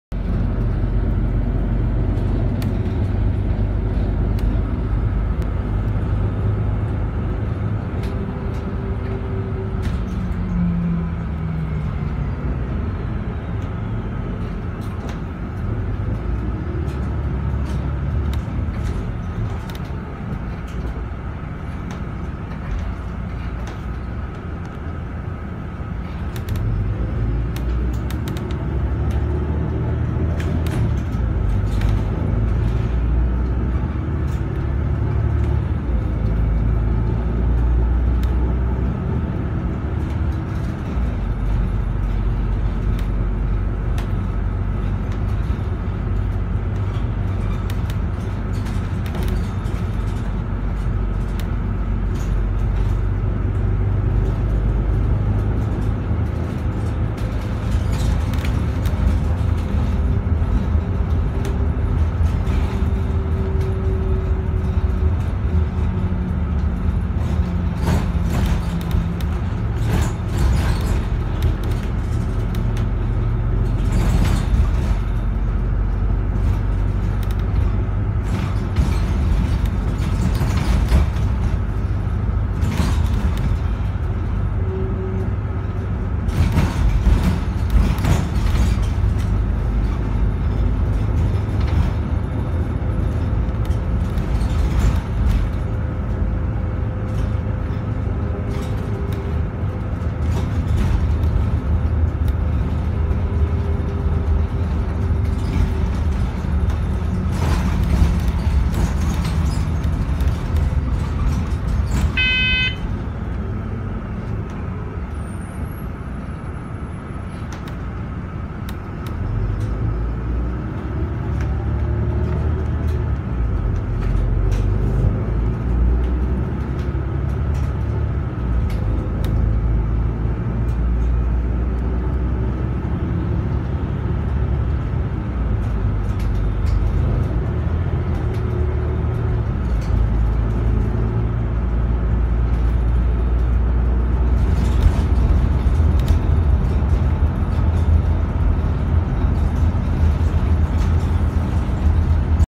Metro Christchurch (Go Bus) MAN 15.250 MCV Evolution [Christchurch, NZ]
Route: 60 (Hillmorton & Wigram - Southshore)
Date recorded: 11 December 2020
Bus #: 1072
Reg: KEB968
Location: New Brighton Rd - Bassett St - Travis Rd, Burwood, Christchurch